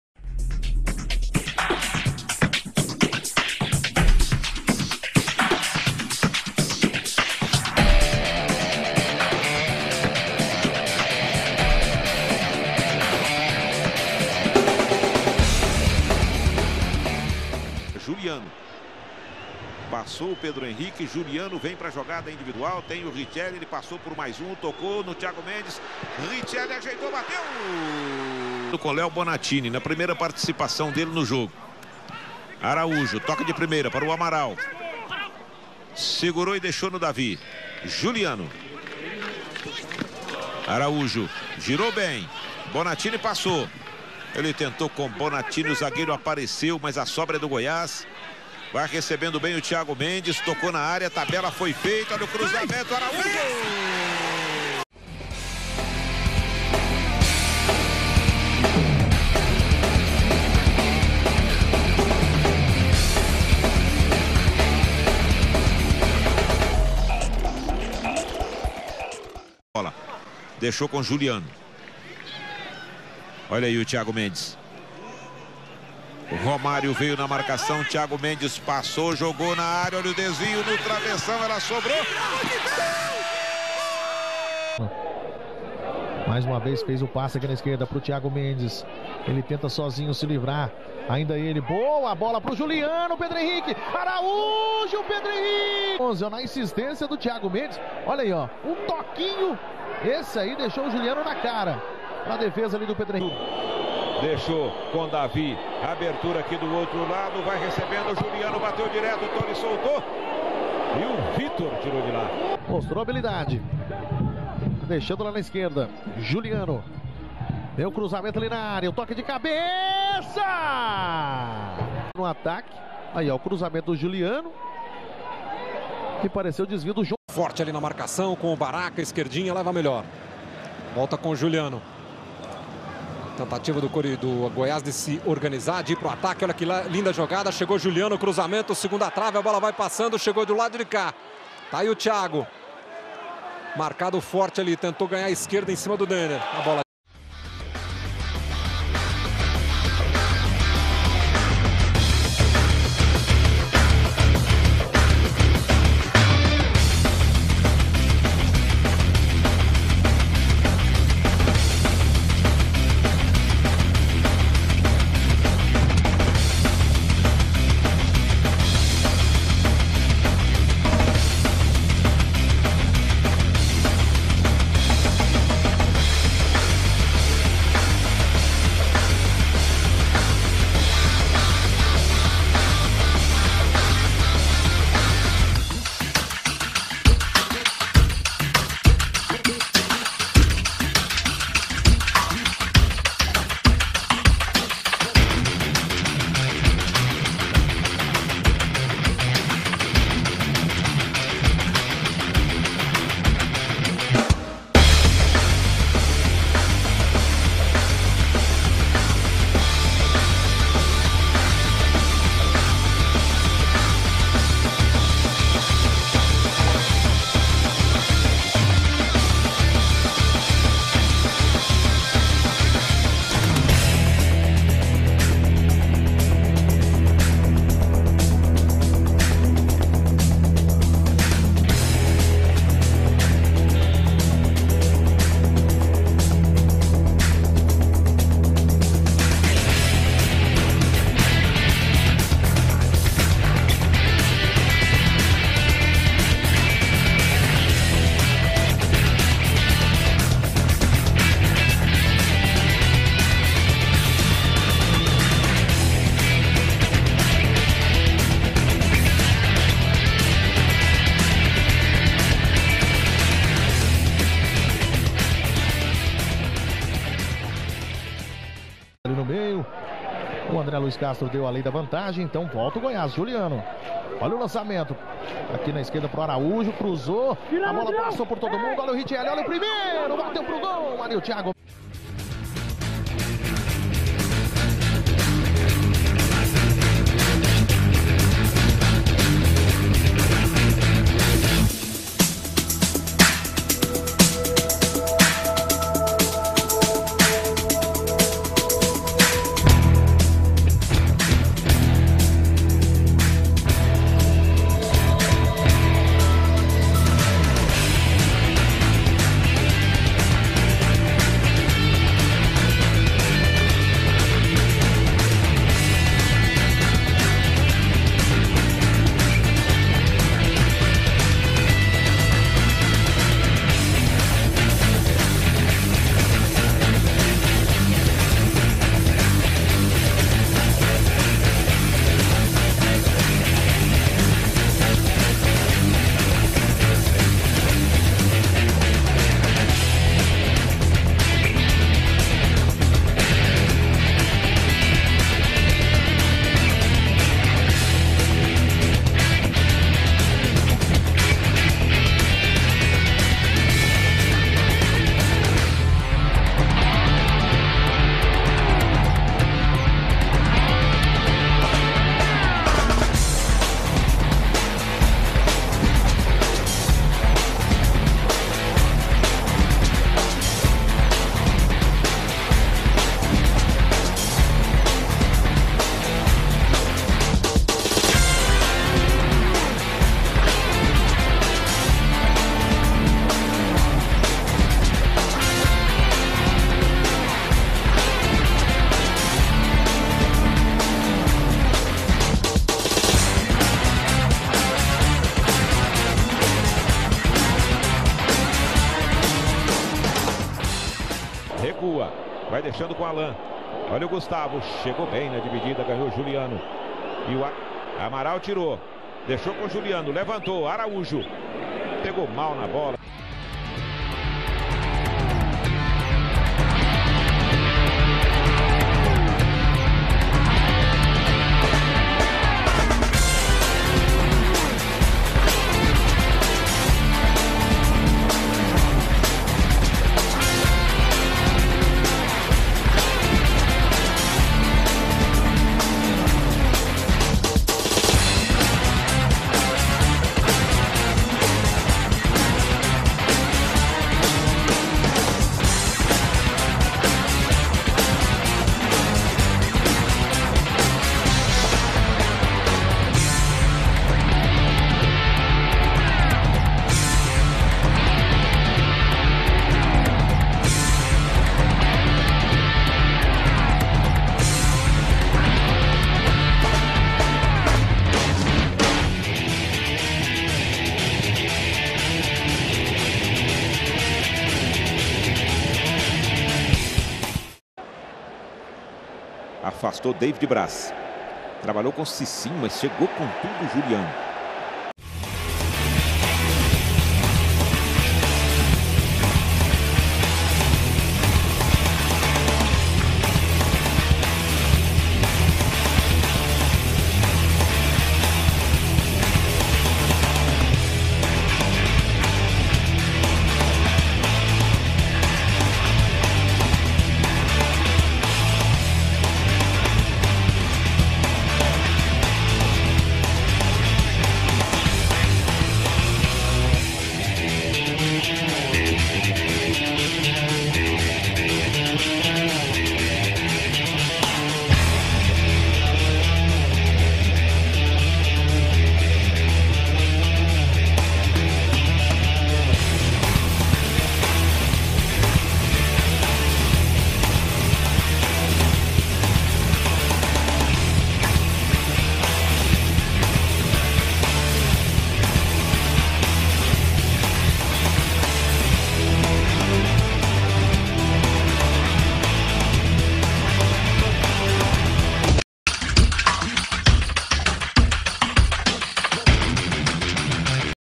Juliano Pacheco
Volante e leteral esquerdo. 1,83m de altura, 79Kg.
Nascido em 06/04/1990
Clubes: Sport Club Internacional
Goisás Esporte Clube